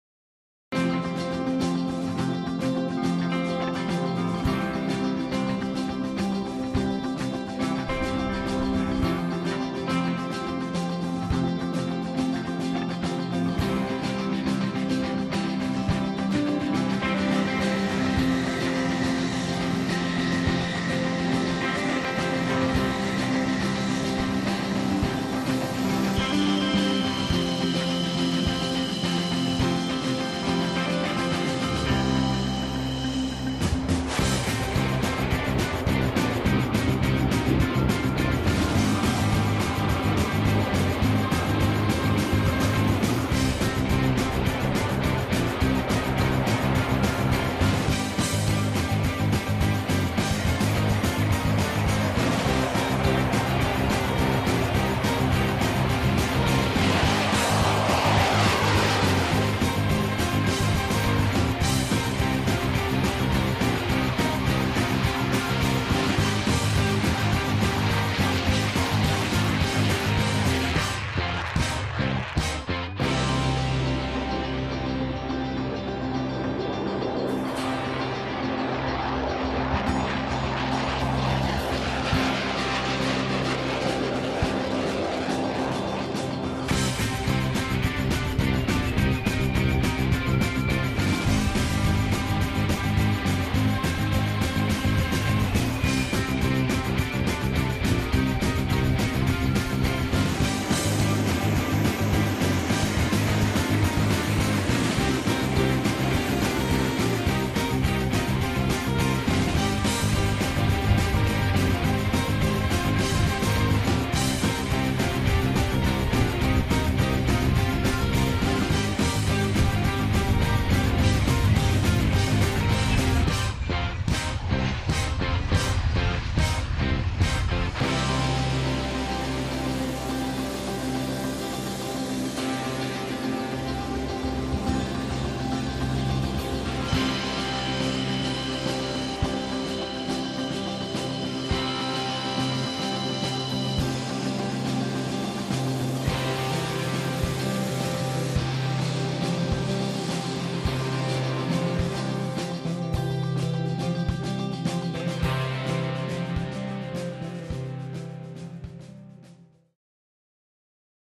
Load Diffuser 2010 gyakorlat
Régen voltak ennyire felpörgetett hétköznapjai az MH 59. Szentgyörgyi Dezső Repülőbázisnak. A kecskeméti légibázison zajlik a Load Diffuser 2010 elnevezésű gyakorlat, amelyen a magyar Gripen és MiG29-es vadászrepülőgépek, vagyis a pumák mellett az Ohioi Nemzeti Gárda F16-osai, azaz a fekete párducok is részt vesznek.